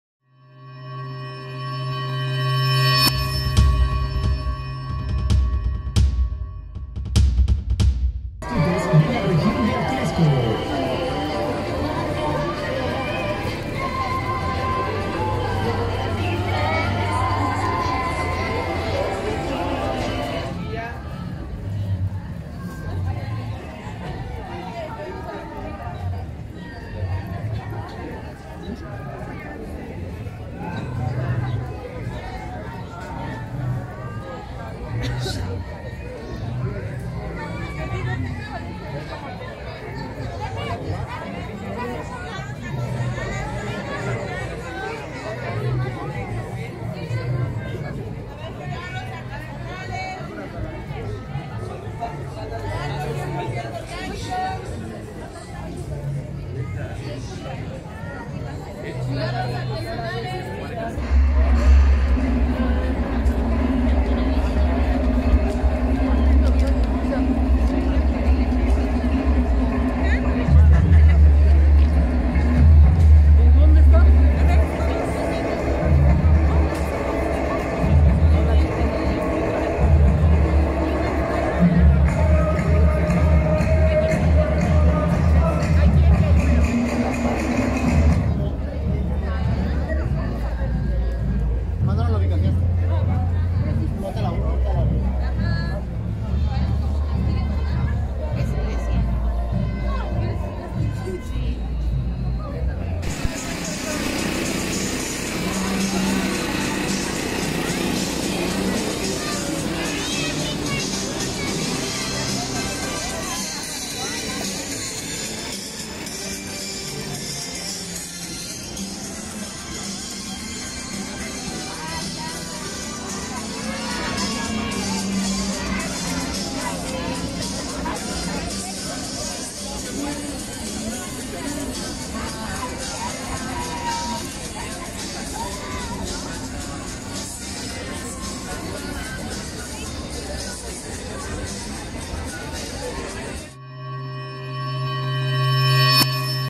Así luce la Villa Iluminada 2019, en Atlixco Puebla
Así luce la Villa Iluminada 2019 de Atlixco Puebla, la cual está disponible desde el 22 de noviembre hasta el próximo 6 de enero de 2020, la cual podrás disfrutar de un largo recorrido.